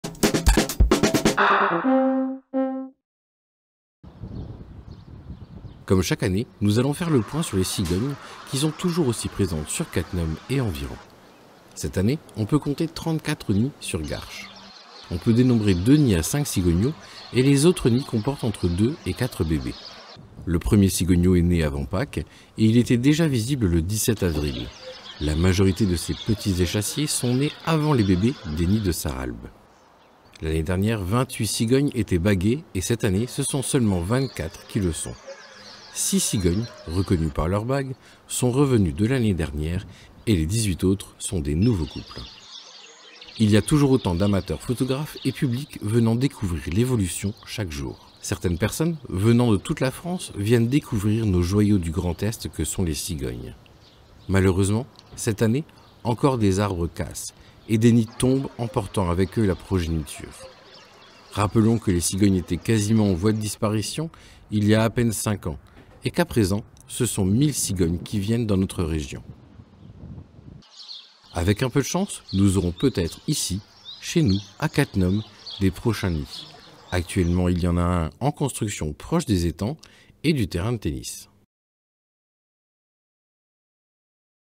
Des cigognes à Cattenom ?
Aussi commun que les pigeons, les cigognes sont très présentes en ce moment sur notre territoire. Les cigogneaux sont nés à Garche il y a peu de temps. Dans un avenir proche nous aurons nous aussi des nids sur Cattenom !